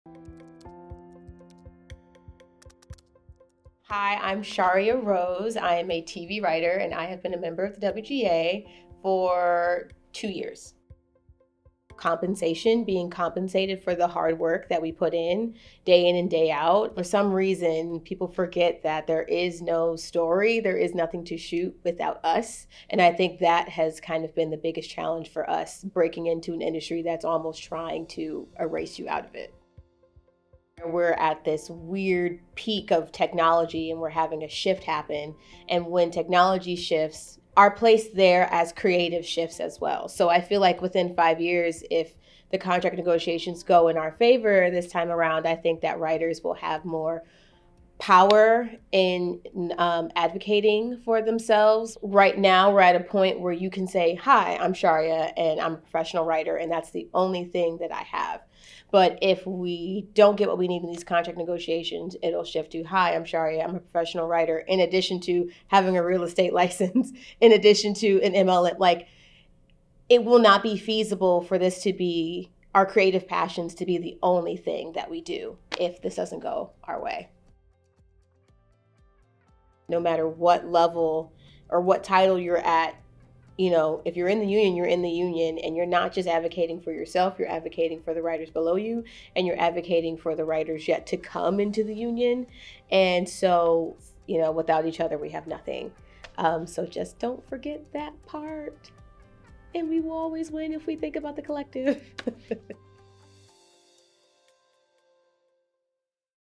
My Union, My Story: Charia Rose
For some reason, people seem to forget that there is no story, there is nothing to shoot without us," says WGA member Charia Rose.